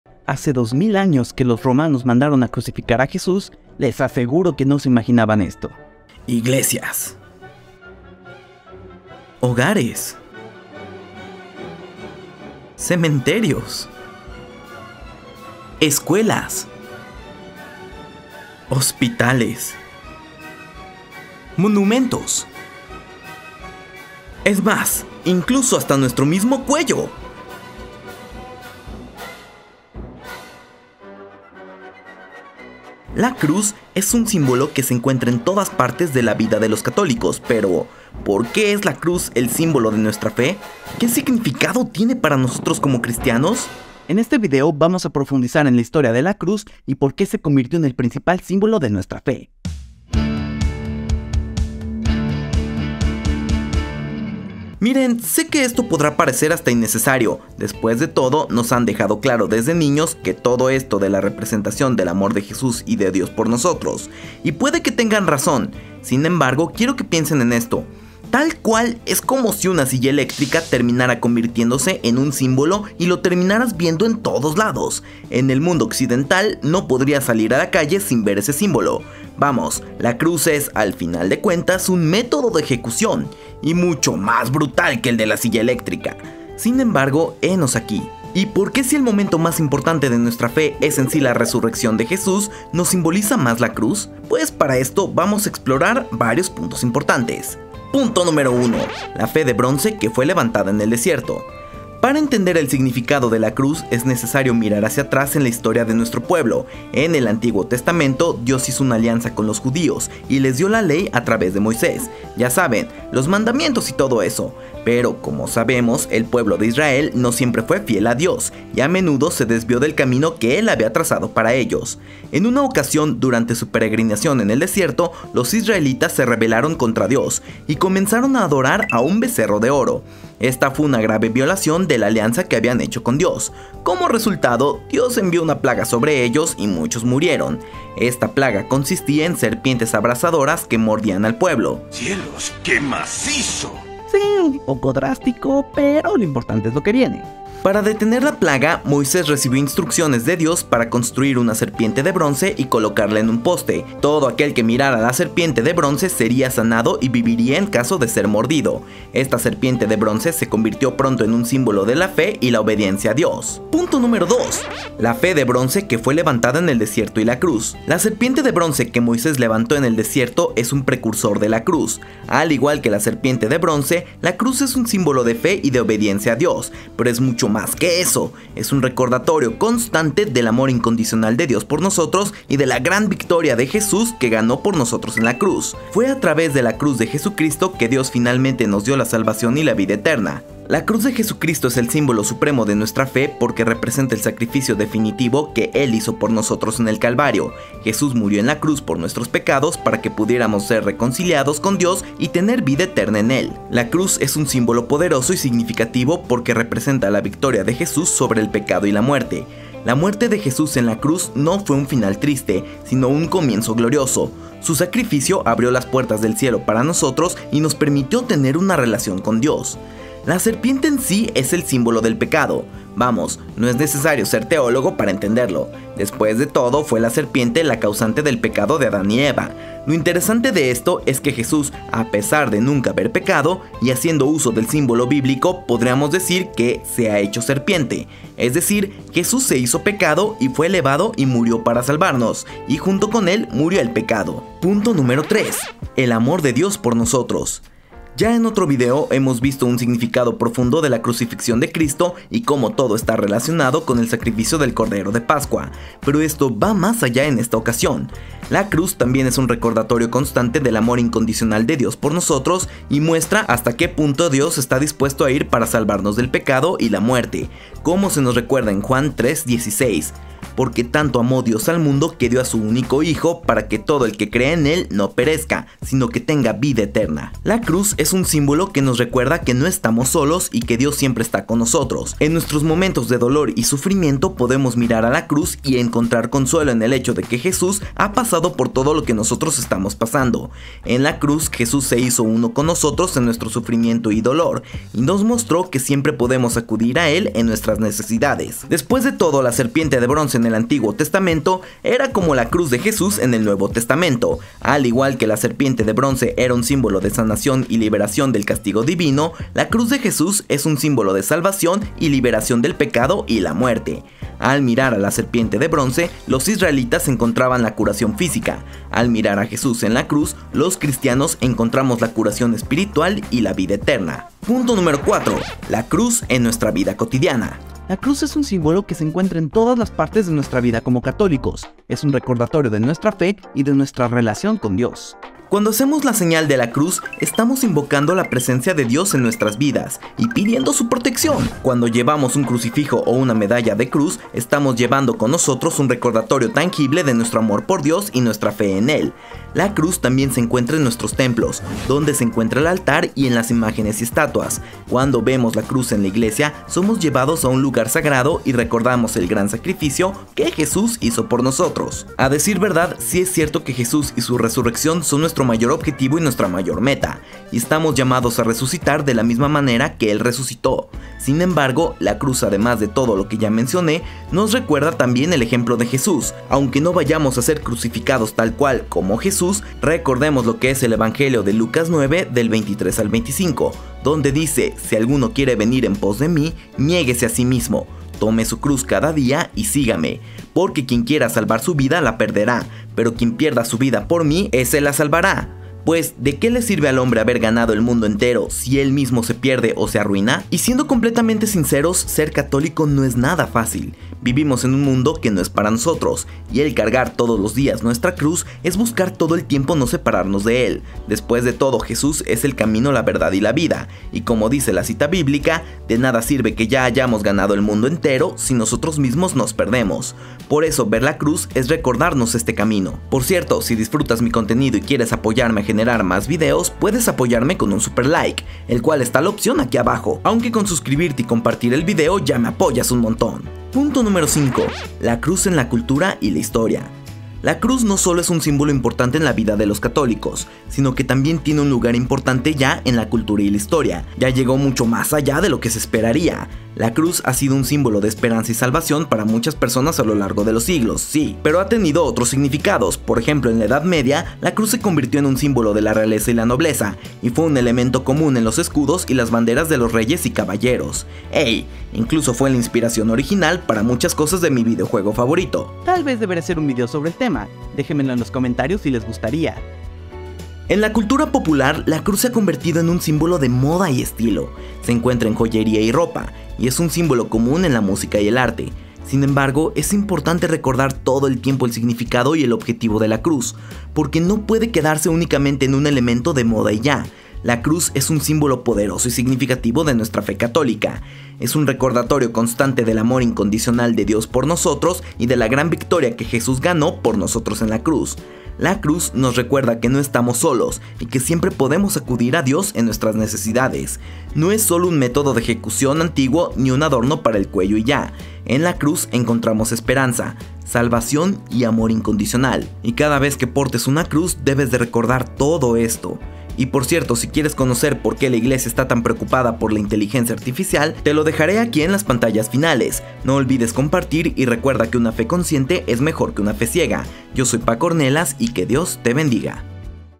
La historia detrás de LA CRUZ de Jesús ✝✝✝
"¿Por qué los CATÓLICOS usamos una CRUZ como símbolo de nuestra fe?" a través de una explicación detallada de la crucifixión de Jesús y el significado simbólico de la cruz en la fe católica. El video explora cómo la cruz se ha convertido en un símbolo central de la fe católica y cómo su uso puede inspirar a los católicos a reflexionar sobre la pasión y muerte de Jesús. Además, el video destaca la importancia de la cruz como un recordatorio del amor y sacrificio de Jesús por la humanidad.

Catecismo de la Iglesia Católica del 571 al 573, del 613 al 617 y del 622 al 623

Si te los perdiste te dejo los últimos videos que seguro son de tu interés.